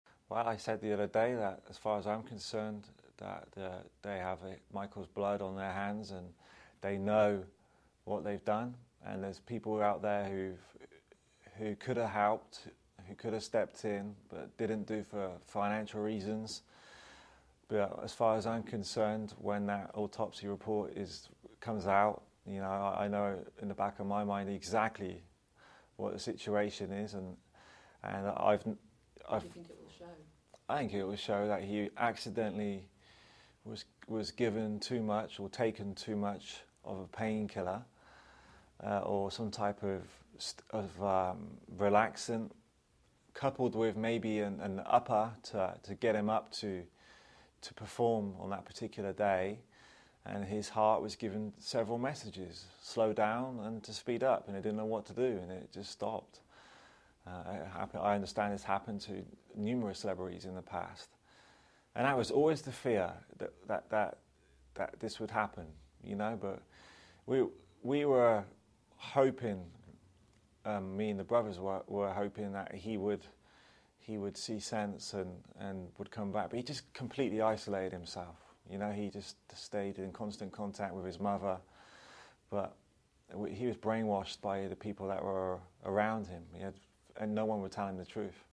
Michael Jackson UK Bodyguard Matt Fiddes Tells Of Star's Secret Girlfriend And Drugs Part III
"As far as I'm concerned they have Michael's blood on their hands, they know what they've done and there's people out there who could have helped, could have stepped in but didn't for financial reasons.
"We went to great efforts to keep the doctors away. But as soon as we said anything and it gets back to Michael, Michael would have a screaming fit that we were interfering with his private life, that he knew what he was doing and he was in denial."